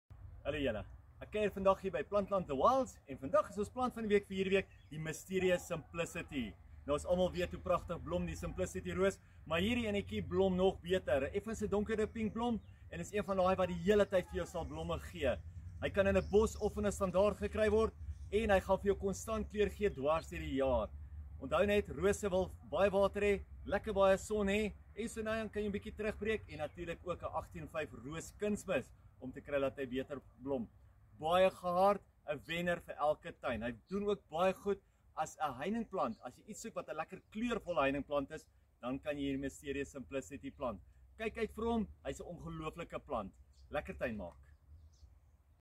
Plant van die week vir 2 Oktober 2021 is die Mysterious Simplicity
Ek was lanklaas so opgewonde oor 'n roos as die plant van die week. Ons plant van die week vir hierdie week is die nuwe Mysterious Simplicity. Ons almal weet dat die Simplicity 'n baie goeie naam het omdat hy so baie blom. Die nuwe een blom nog meer e  die blomme is effens groter en donkerder pienk as die gewone Simplicity. Beskikbaar in standaarde sowel as bosse en kan so net meer as 'n meter hoog groei. Die plante is baie gehard en waterwys - voer met 8:1:5 en kry die meeste uit jou rose!!